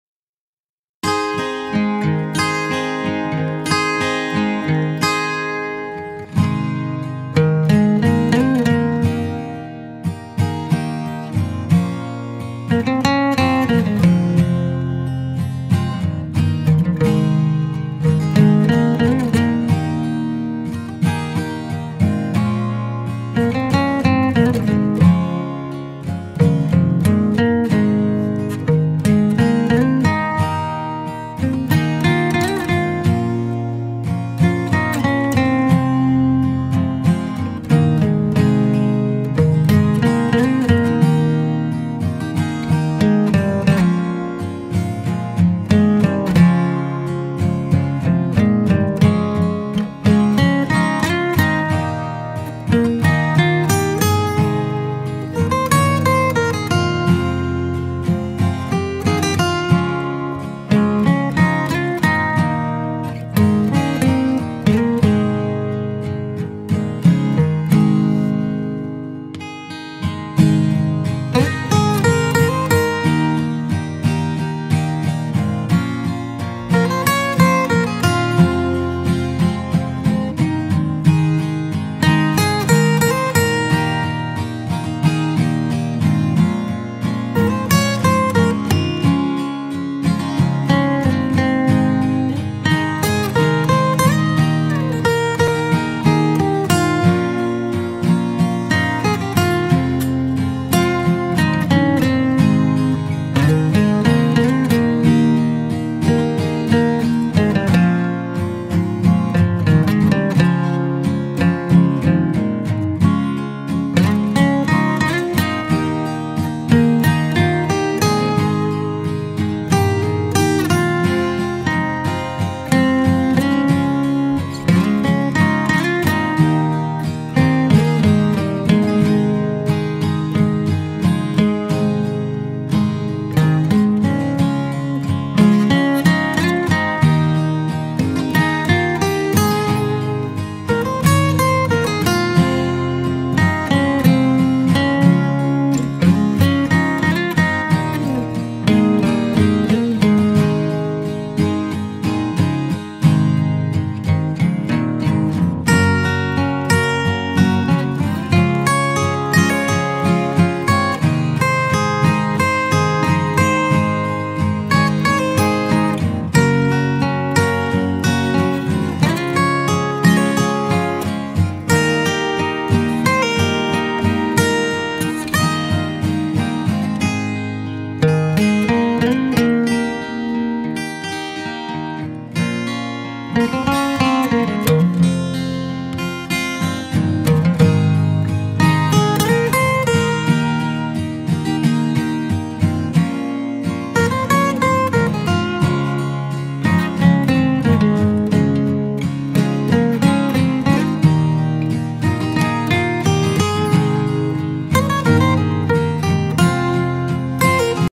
Yavuz Sultan Selim Köprüsü ve Boğaz Manzarası
Yavuz Sultan Selim Köprüsünü boğaza da hakim güzel bir yerden çektim. Bitince güzel olacak.